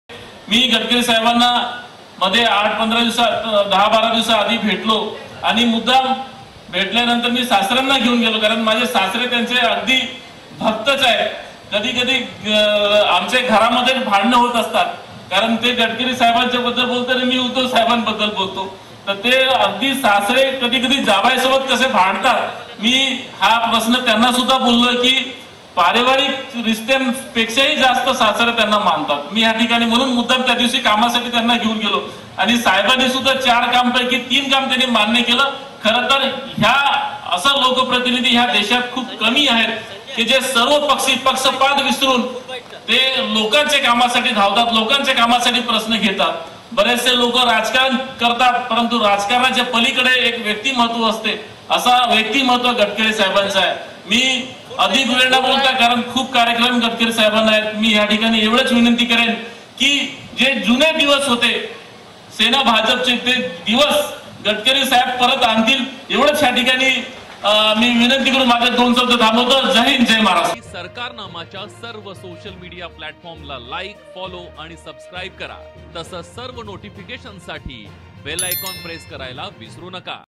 Nitin Gadkari | शिवसेनेच्या आमदाराची युतीसाठी गडकरींनी गळ | Sarkarnama
भंडारा(Bhandara) शहरातून जाणाऱ्या सहा पदरी बायपास रस्त्याच्या कामाचे भूमिपूजन केंद्रीय मंत्री नितीन गडकरी(Nitin Gadkari)यांच्या हस्ते झाले. कार्यक्रमा प्रसंगी शिवसेना आमदार नरेंद्र भोंडेकर यांनी जाहीर सभेत ही इच्छा प्रकट करत गडकरी साहेबाना प्रेमळ गळ घातली. ''गडकरी साहेबच भाजप व शिवसेनेचे जुने दिवस आणतील,'' असा विश्वास नरेंद्र भोंडेकर यांनी यावेळी व्यक्त केला.